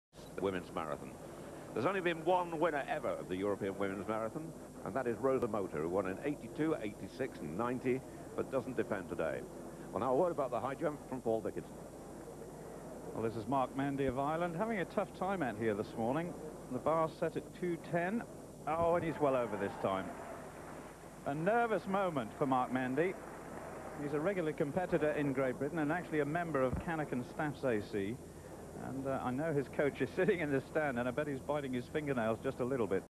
4663 European Track & Field High Jump Mark Mandy
1994 European Helsinki High Jump Men Qualifying Mark Mandy 2m10
1.  Lambros Papakostas (GRE) 2.26 m
2.  Dalton Grant (GBR) 2.26 m
3.  Steinar Hoen (NOR) 2.23 m
4.  Wolfgang Kreissig (GER) 2.23 m
5.  Jarosław Kotewicz (POL) 2.23 m
 Dragutin Topić (FR Yugoslavia)
 Oleg Zhukovskiy (BLR)
8.  Brendan Reilly (GBR) 2.20 m
9.  Roberto Ferrari (ITA) 2.20 m
 Robert Kołodziejczyk (POL)
11.  Sorin Matei (ROM) 2.15 m
12.  Mark Mandy (IRL) 2.15 m
13.  Zoltán Bakler (HUN) 2.10 m